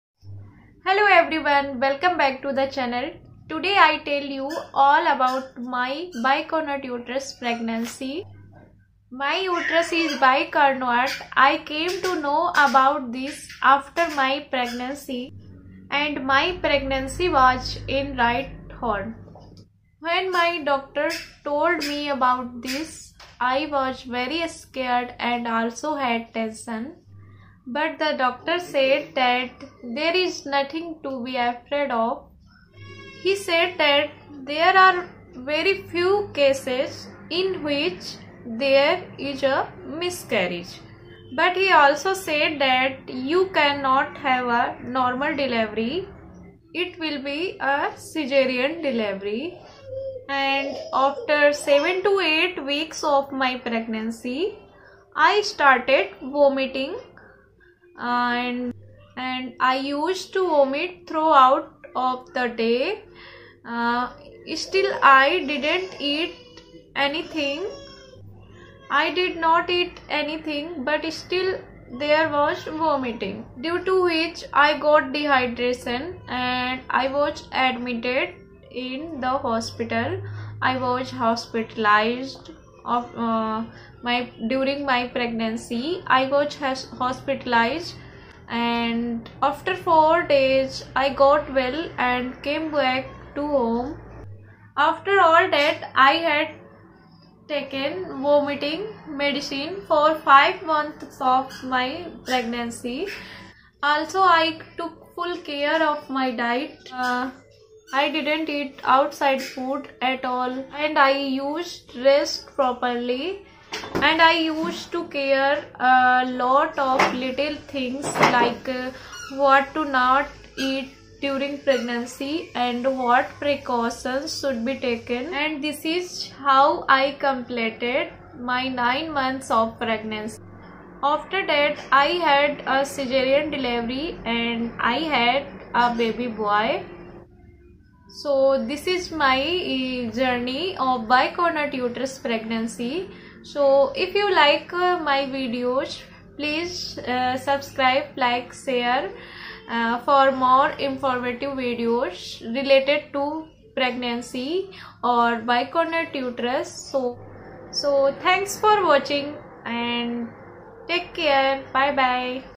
My bicornuate uterus pregnancy in English|| How I completed 9 months of my Bicornuate pregnancy
My bicornuate uterus pregnancy story.  there is all about my In this video.
Hi friends, maine bicornuate uterus se related  bahut sari videos upload kar di h mere channel par plz ek bar visit kare aapke sabhi doubts clear ho jayege.